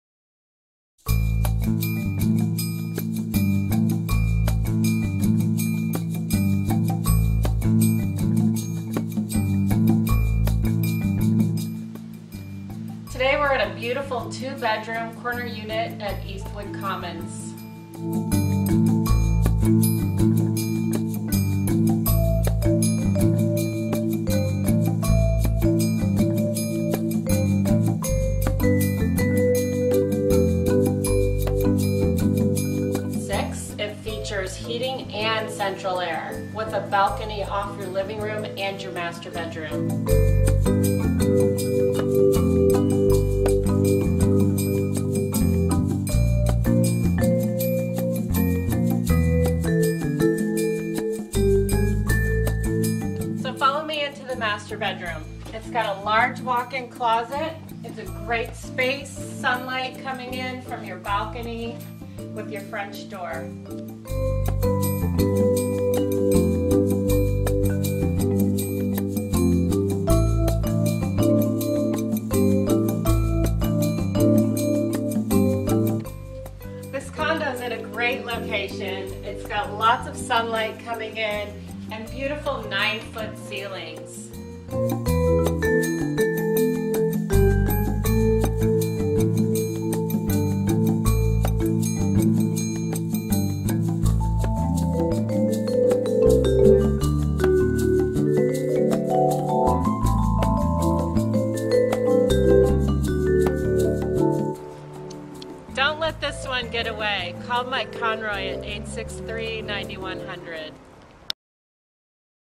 370 Farrell St. #308, South Burlington
Best 2 bedroom Condo in the building! Convenient location walk to shopping, bike path, bus line, restaurants, a quick drive to the beach and Church St. Deck off master suite and living room.  Nice secure lobby, exercise room, underground parking, elevator.  Come check it out for yourself.